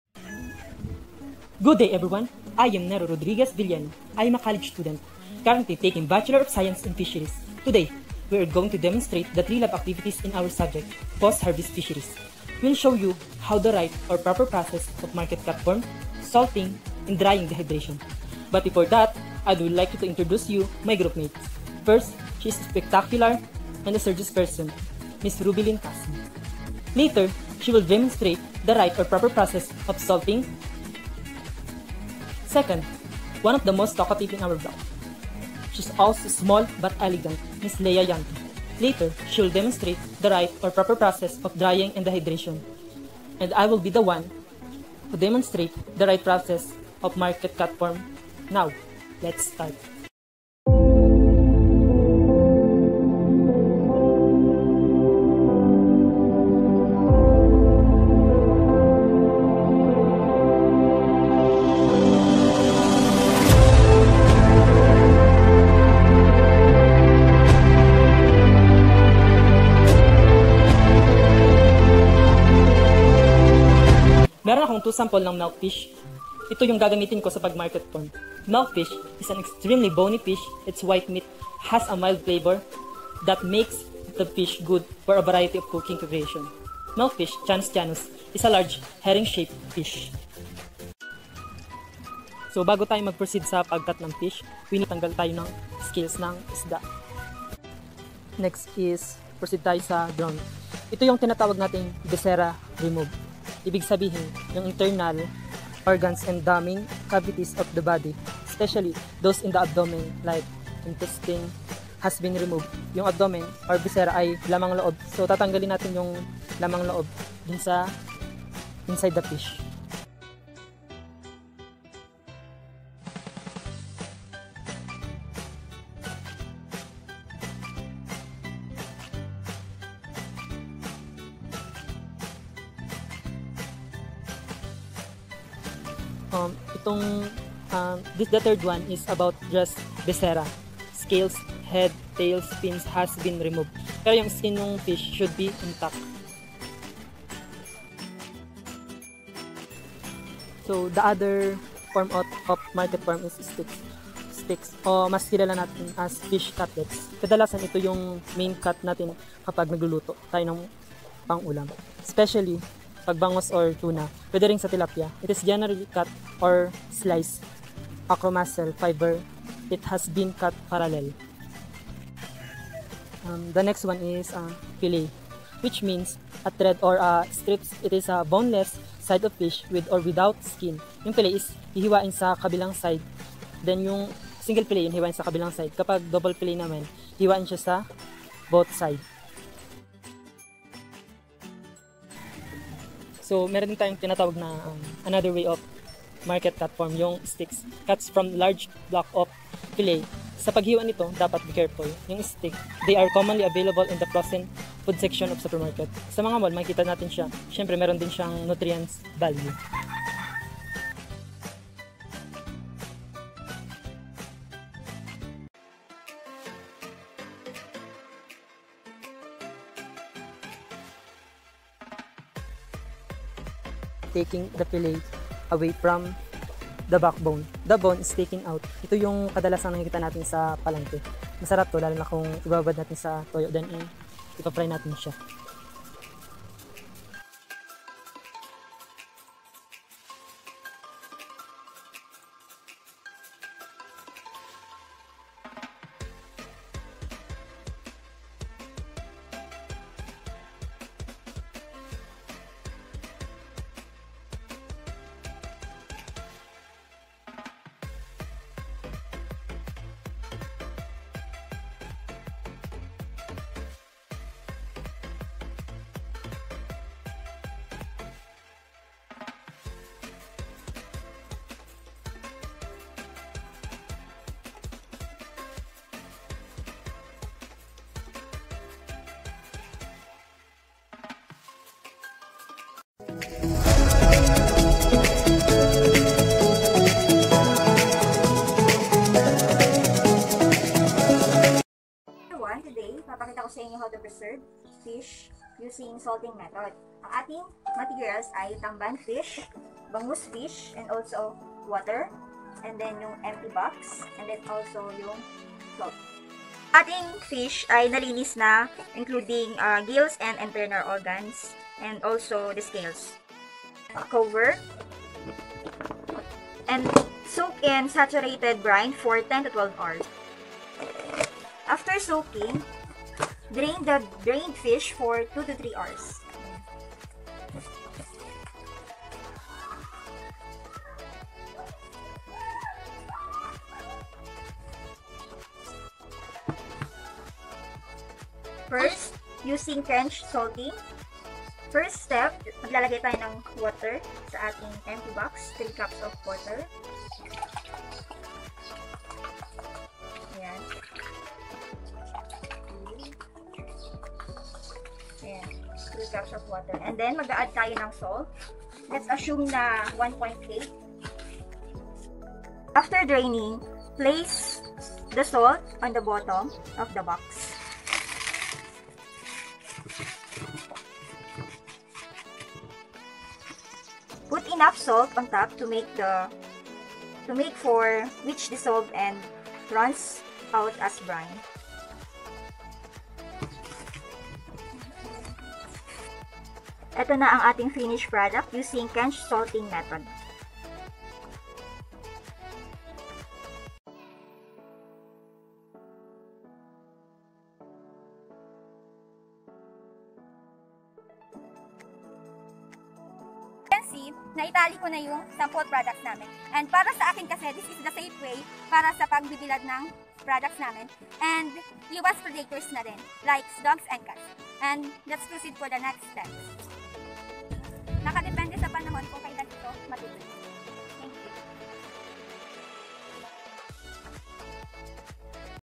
Post-harvest fisheries subject: Lab activities Market cut form | Salting | Drying and Dehydration
Be sure that the seafood you buy is fresh. It’s a good idea to go to the market to buy fresh seafood, not just a particular species. If your recipe calls for flounder, but the snapper is fresher, buy the snapper